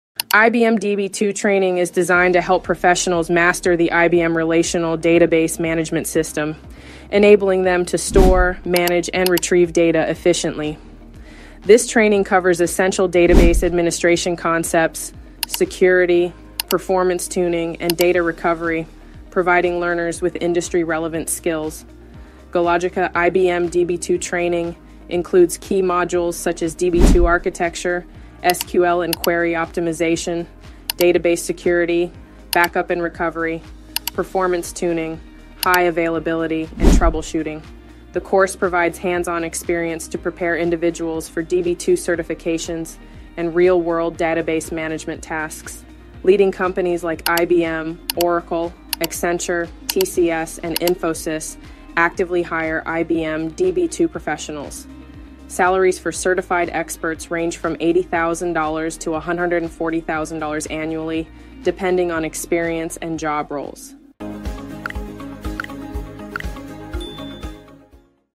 🔥 IBM DB2 Training | Master Database Management with GoLogica 🚀
🔥 IBM DB2 Training | Master Database Management with GoLogica 🚀 ibmdb2 gologica db2training databasemanagement sql ibmdatabase #db2certification
Want to become an expert in IBM DB2 Database Management? 🚀 The GoLogica IBM DB2 Training equips you with the skills to manage, optimize, and secure enterprise databases efficiently.
🔹 What You’ll Learn:
✅ DB2 Architecture & Installation
✅ SQL Queries & Optimization
✅ DB2 Security & Performance Tuning
✅ Backup & Recovery Strategies
✅ Hands-on Labs & Real-World Scenarios

Instagram:  :-   / gologicatechnologies
Facebook:  :-   / gologica
 LinkedIn :  :-   / mycompany